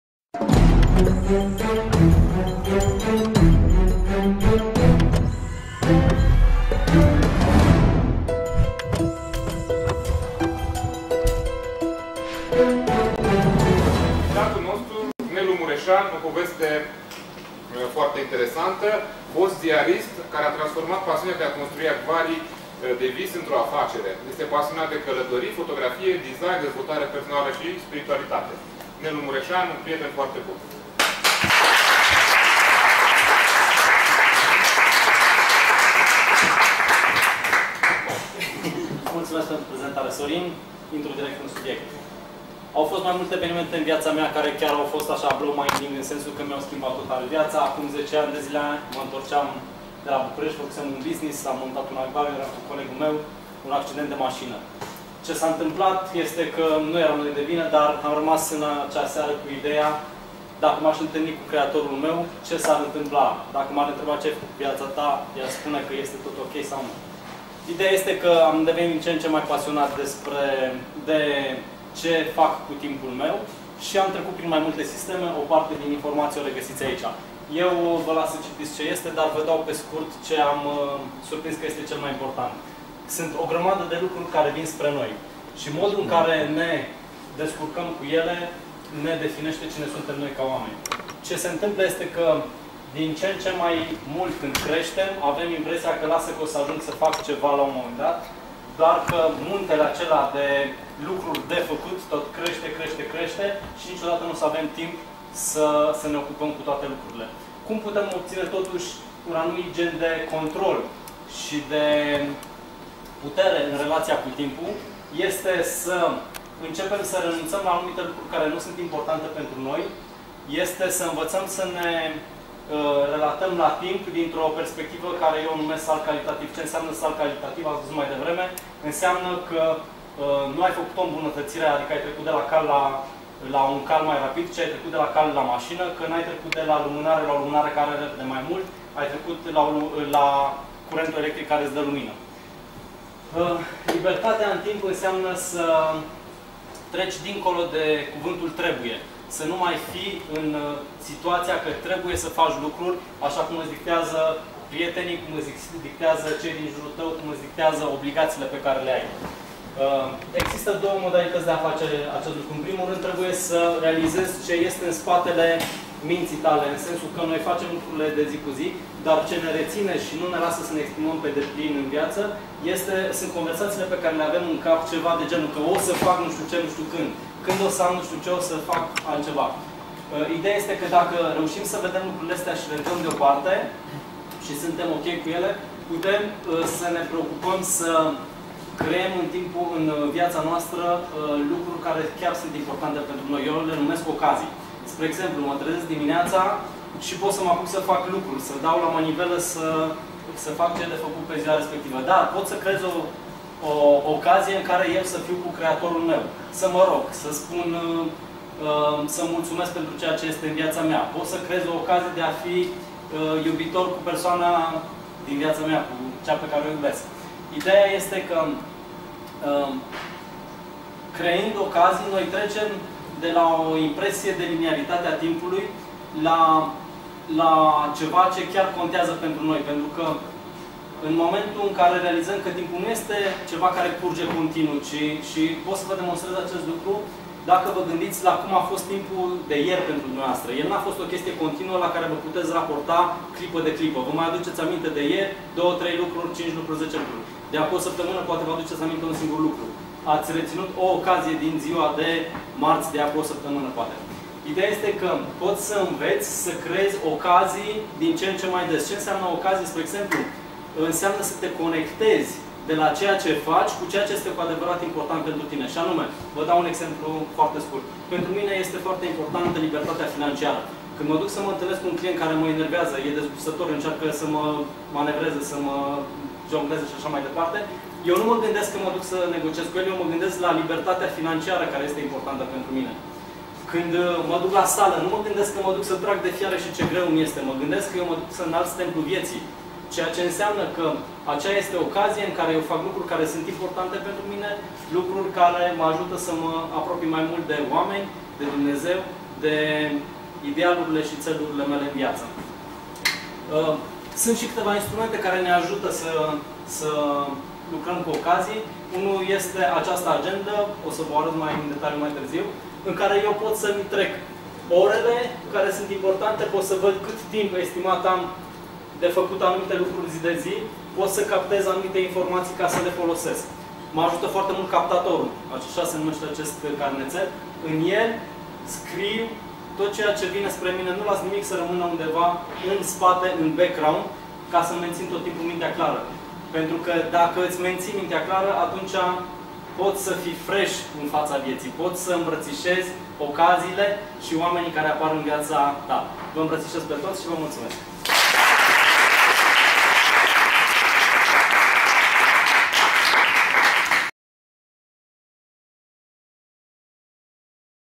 Ioan Muresan- Ce ai facut cu viata ta?
Ioan Muresan a fost invitatul special al editiei cu numarul 3 5MS din Brasov si ne-a invatat cum sa obtinem control in relatia cu timpul!